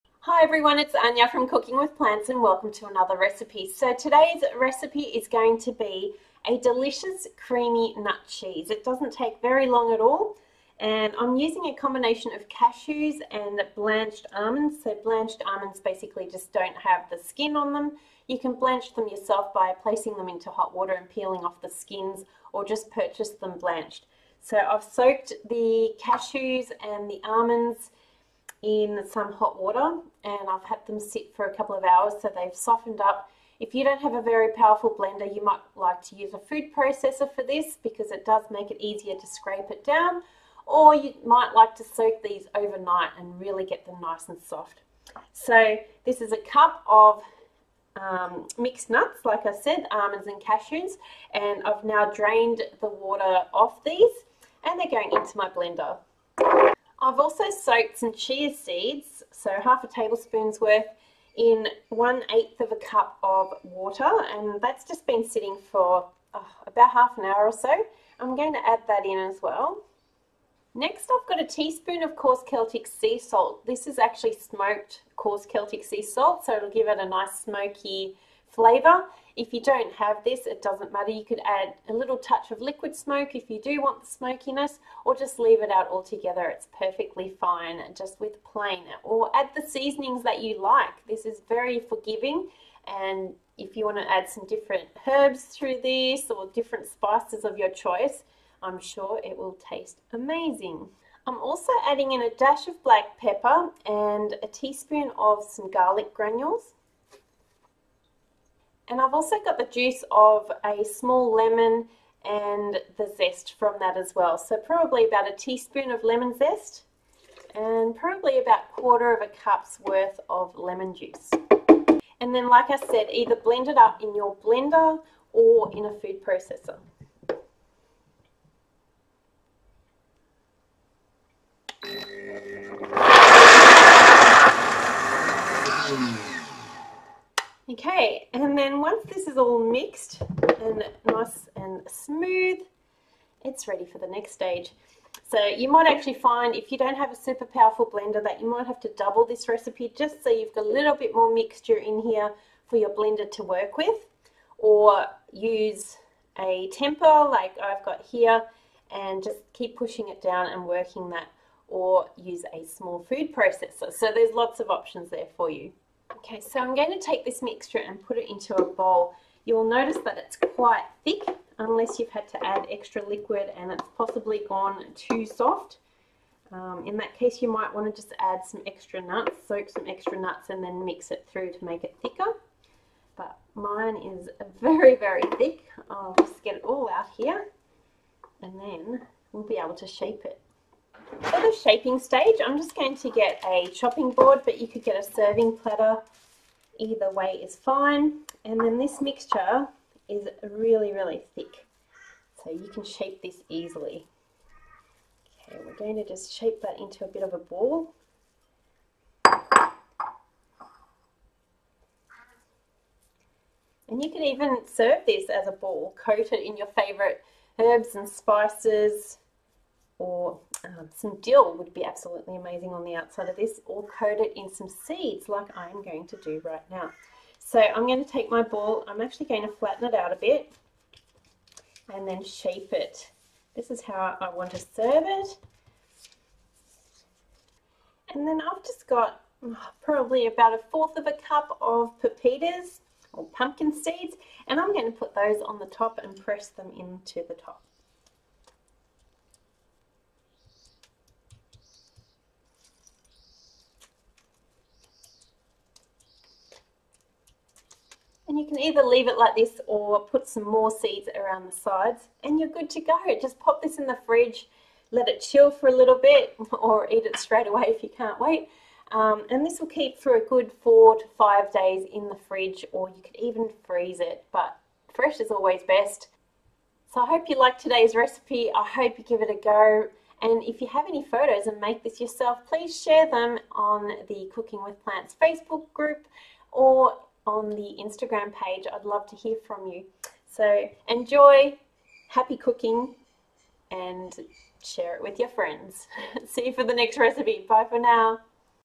Vegan Cream Cheese Wheel Recipe - Spreadable & Creamy!
This easy vegan cream cheese recipe is great to serve as part of a fruit and crackers board! Ideal for parties or just as a quick vegan cheese to have as an every day go-to.  This vegan cream cheese wheel only uses a few basic ingredients and is super quick and easy to make. It’s creamy, spreadable and tastes indulgent.

Don't know how to get started on a plant based vegan lifestyle?

👉Check out all of my recipes, cookbooks, online cooking classes and more